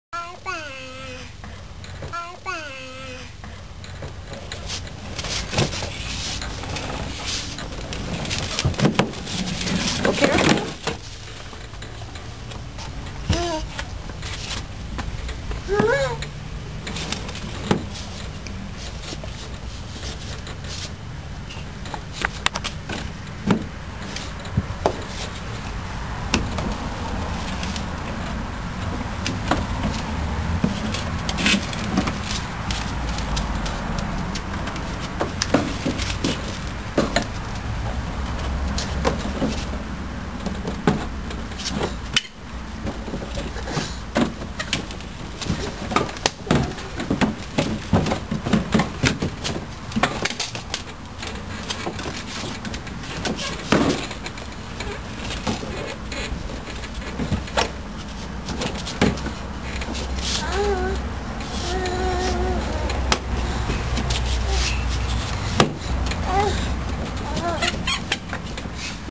Riding shot gun
Peyton decided she wanted to ride on the passengers side.